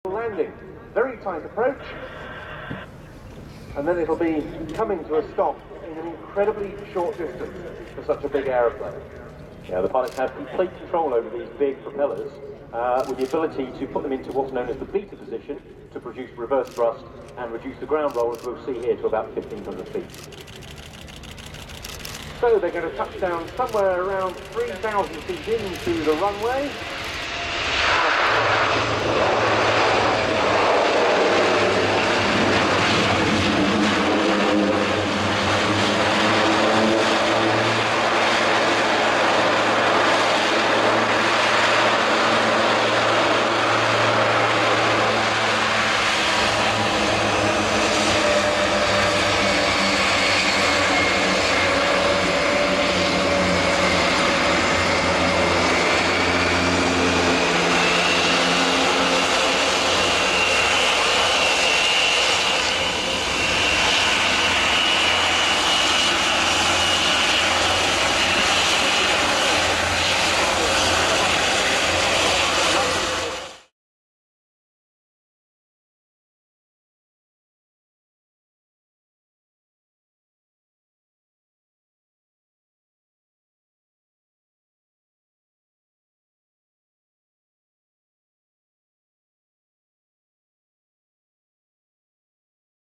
Airbus A400M Atlas Combat approach Short Landing and reverse RIAT 2019 RAF Fairford AirShow
Airbus A400M Atlas Combat approach Short Landing and reverse at the Royal International Air Tattoo at RAF Fairford
RIAT flying Display on Sunday 2019
Reg : EC-400
MSN : 056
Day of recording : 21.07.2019